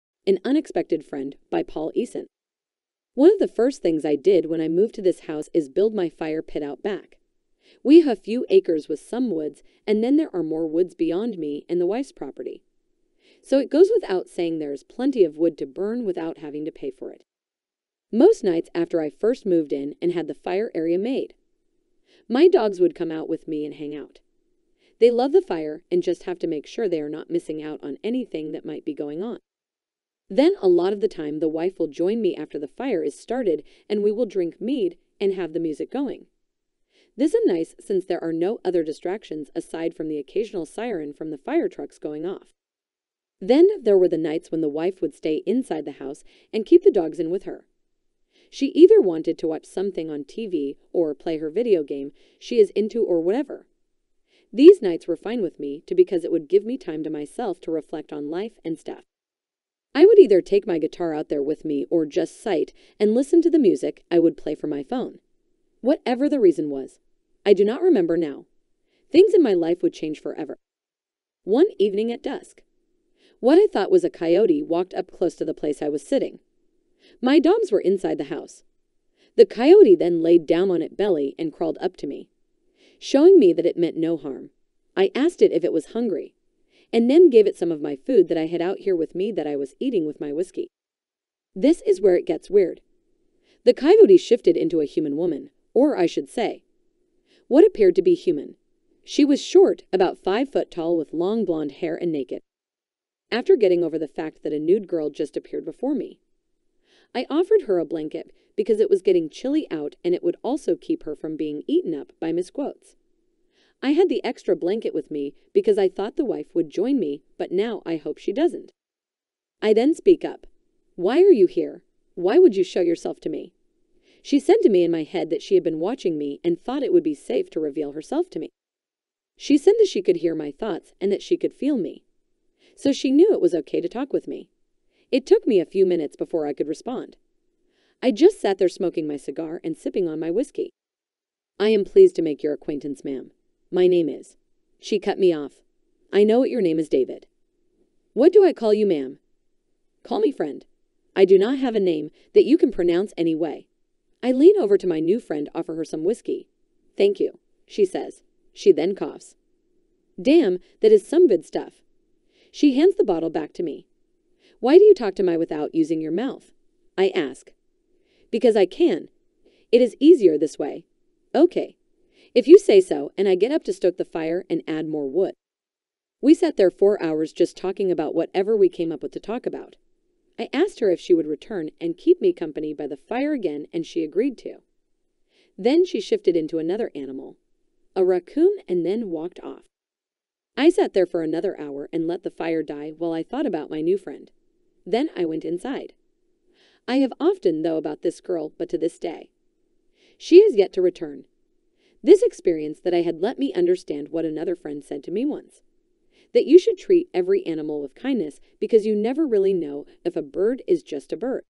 An Unexpected Friend by Paul Ieson
A Short story I wrote and put it into a video.
Story is copy writen by Paul Ieson 2021